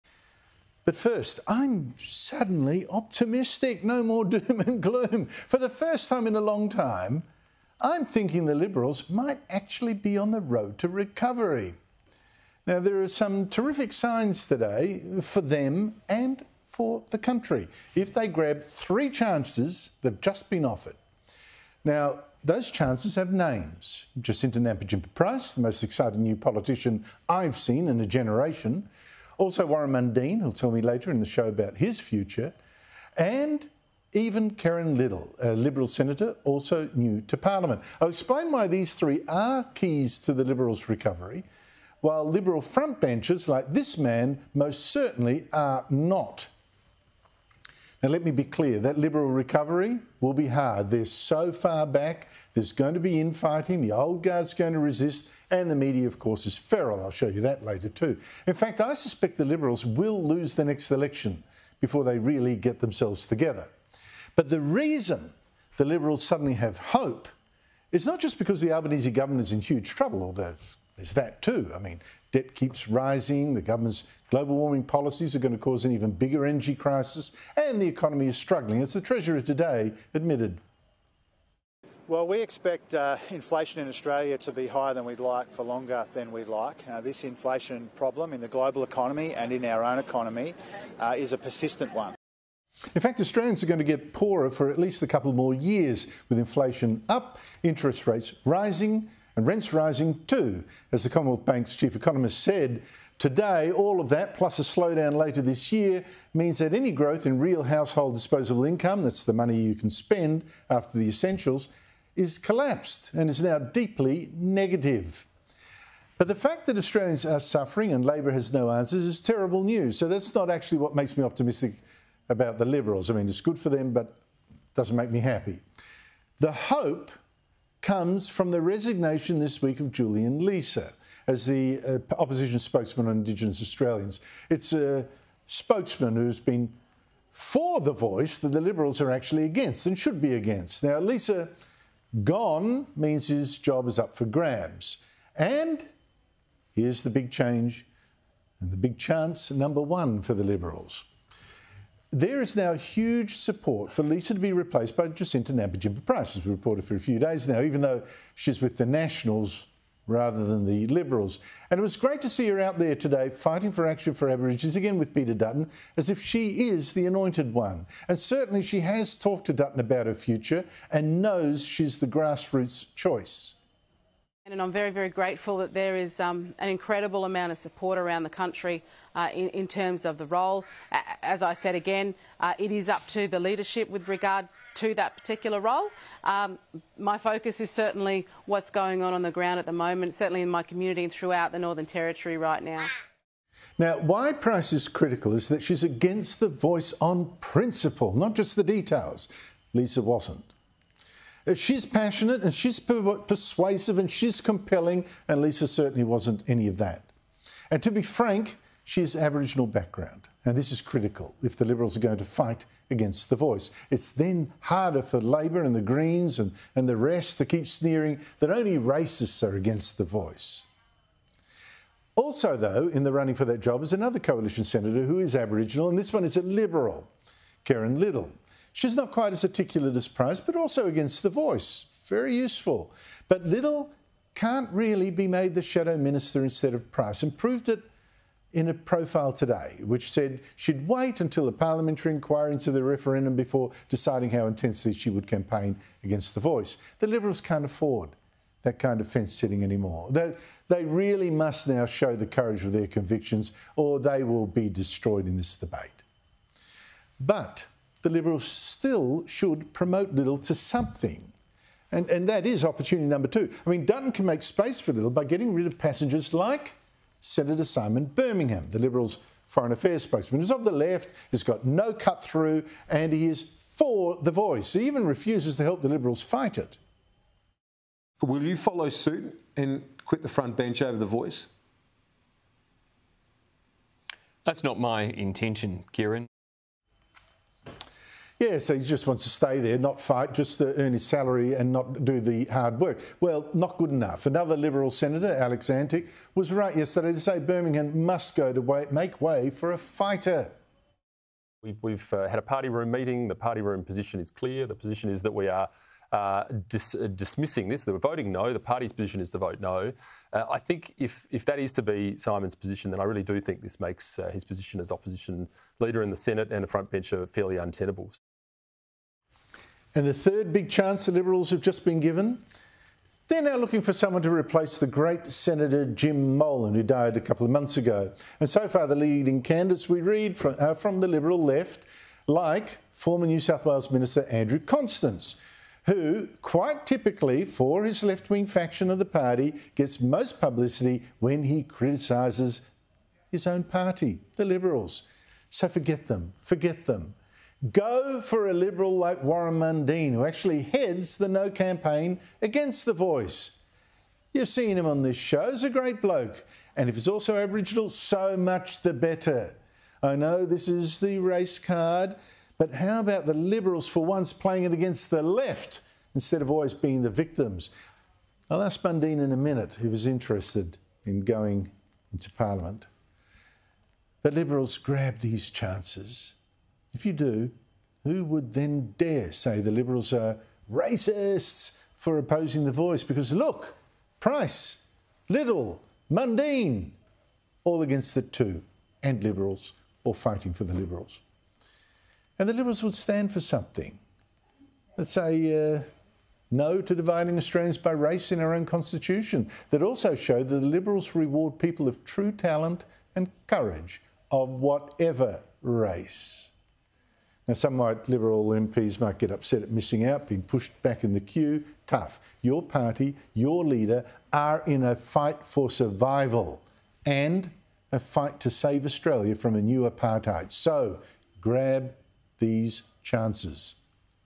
Coalition might be on the ‘road to recovery’: Andrew Bolt
Sky News host Andrew Bolt thinks the Coalition might actually be on the “road to recovery” after some terrific signs on Thursday.

“Jacinta Nampijinpa Price, the most exciting new politician I've seen in a generation, also Warren Mundine … and even Kerrynne Liddle, a Liberal senator also new to parliament,” he said.

Mr Bolt said the three politicians are key to the Liberal Party’s recovery while frontbenchers like Foreign Minister Simon Birmingham are not.

“(Julian) Leeser gone means his job is up for grabs and here’s the big change and big chance number one for the Liberals.

“There is now huge support for Leeser to be replaced by Jacinta Nampijimpa Price … even though she's with the Nationals rather than the Liberals, and it was great to see her out there today fighting for action for Aborigines again with Dutton as if she is the anointed one.”